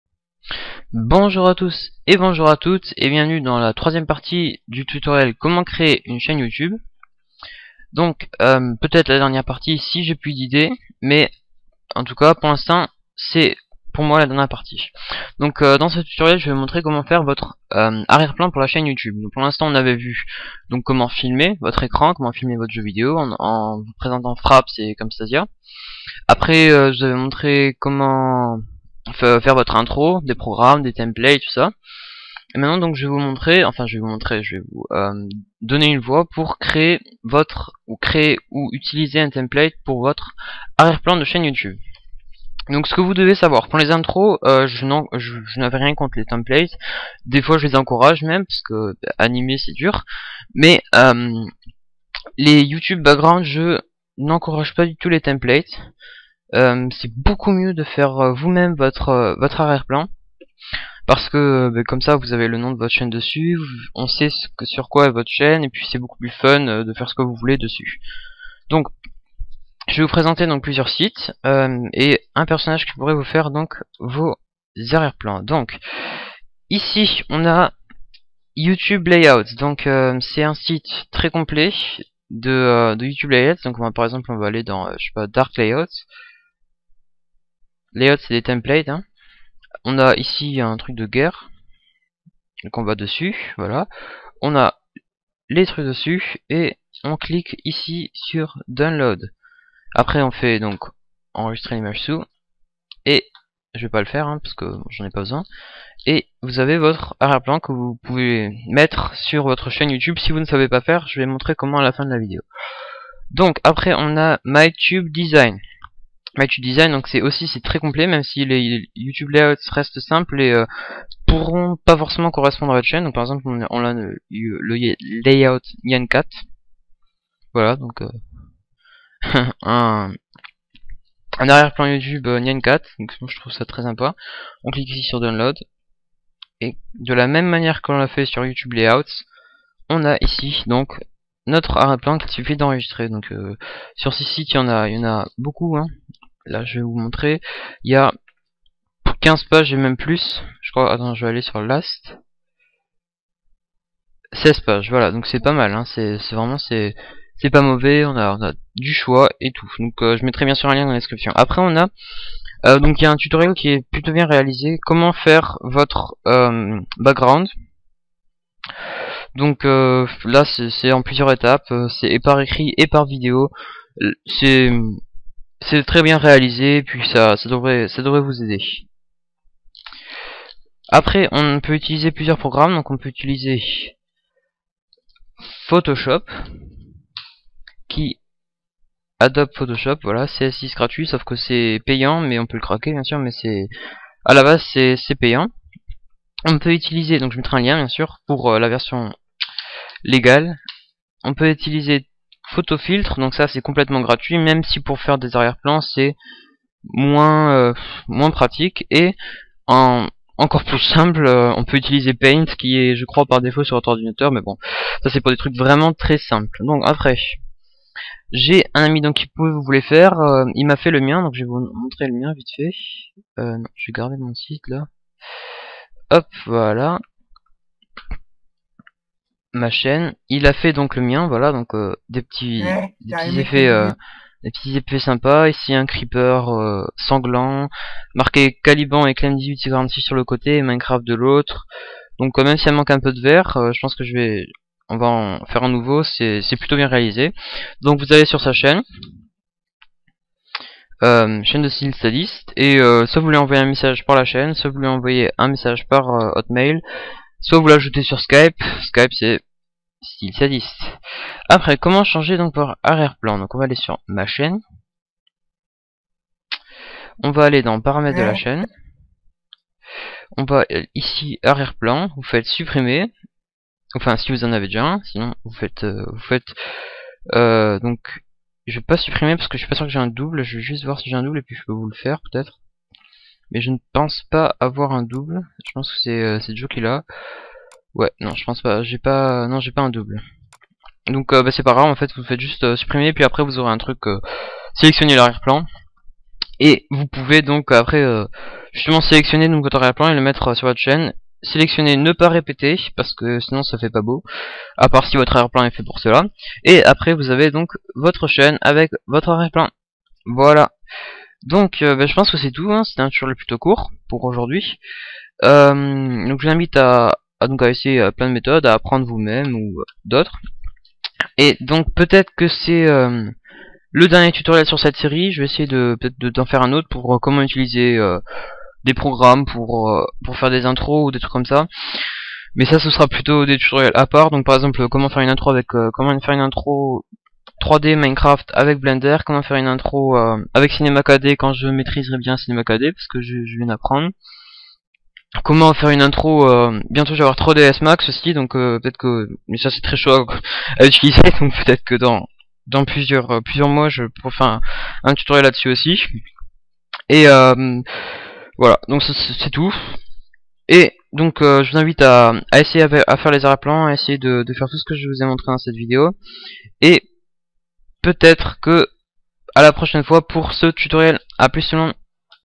Calicraft Tuto HD: Créer une chaîne youtube sur les jeux vidéos. Partie 3: Youtube Background
Voici la troisième et peut-être dernière partie de la série tutoriel : Comment créer une chaîne youtube sur les jeux vidéos. Dans cette partie je vous montre comment créer un arrière plan youtube, ou plutôt comment en avoir un/ comment apprendre à en faire un.